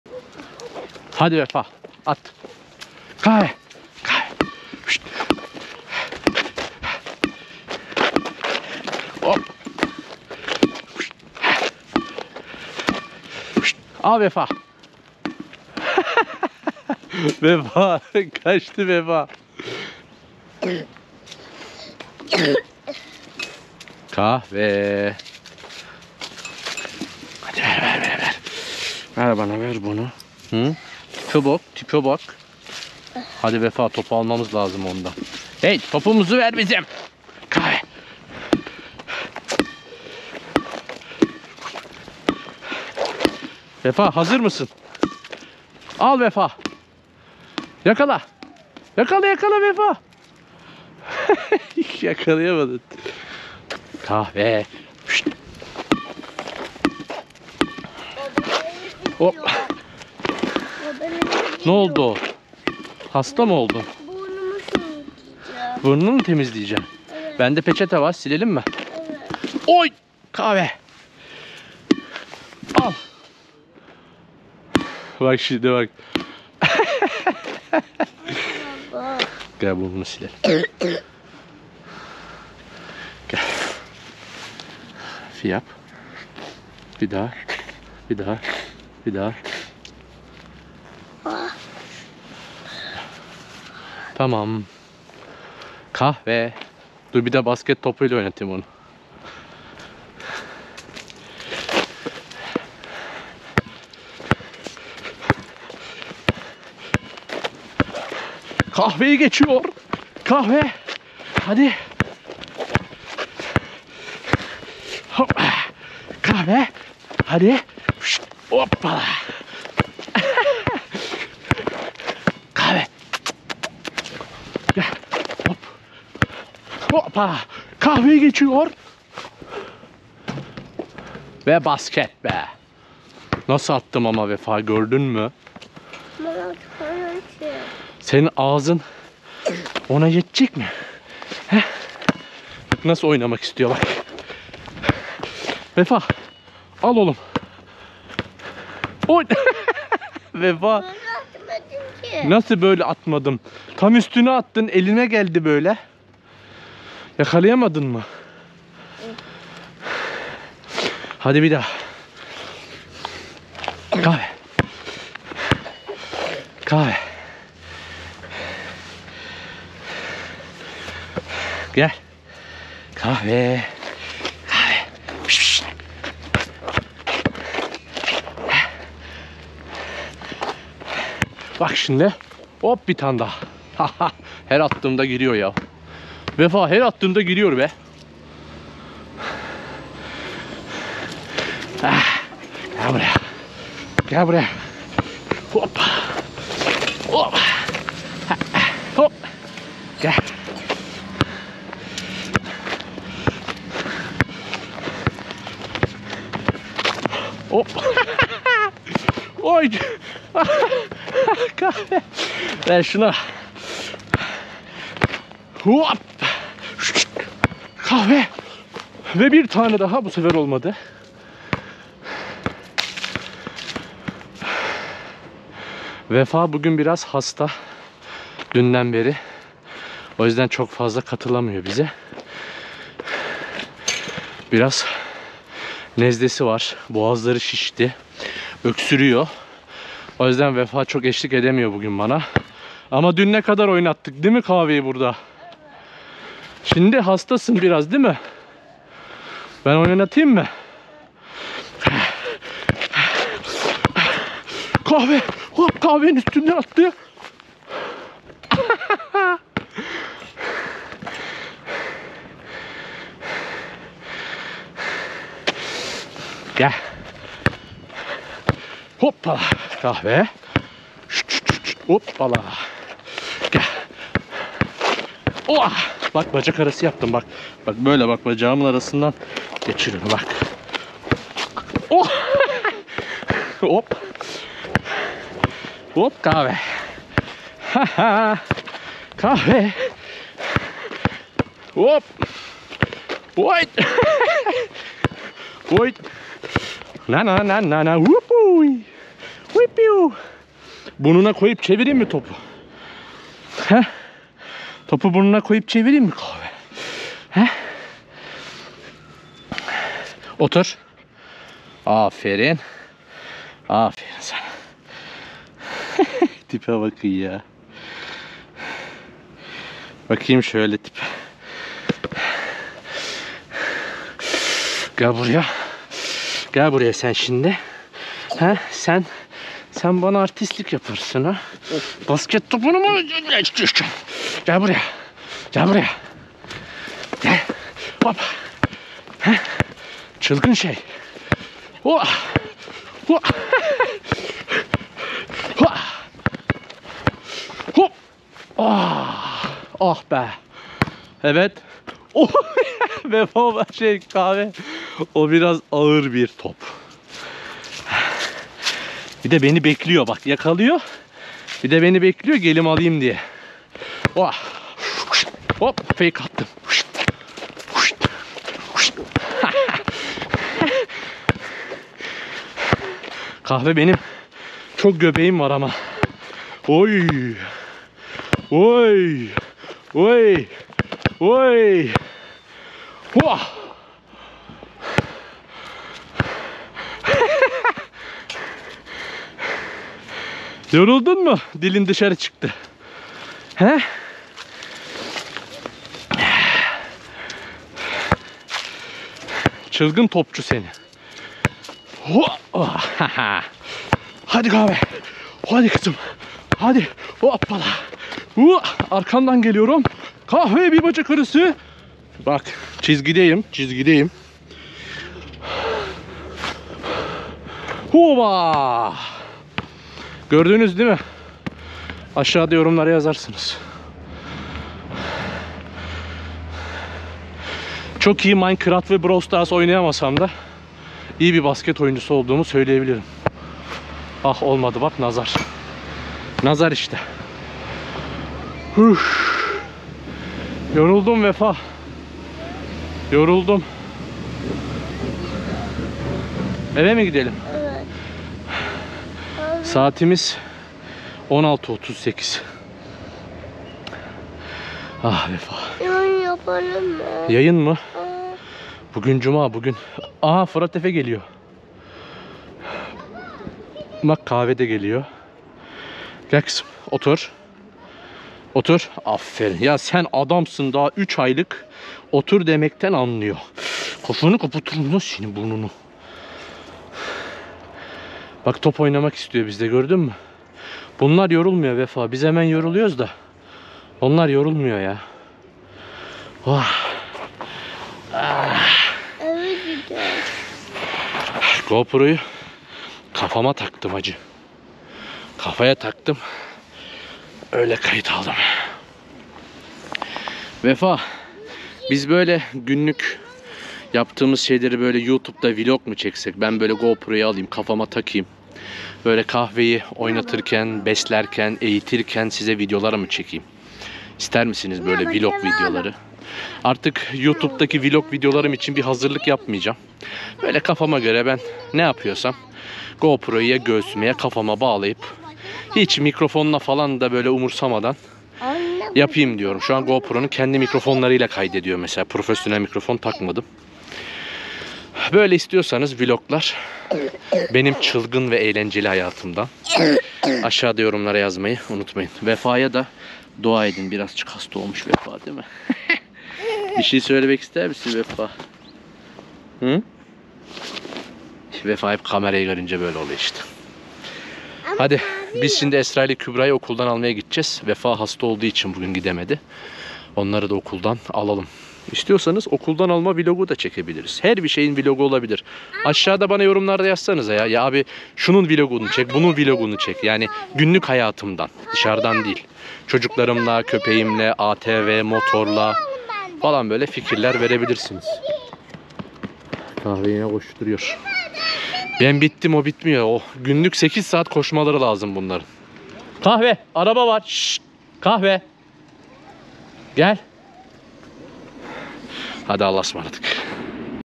SERTAÇ ABİ'NİN KAHVESİ (Border Collie Puppy Yavru Köpek)
Tanıştırayım ailemizin yeni üyesi Kahve :)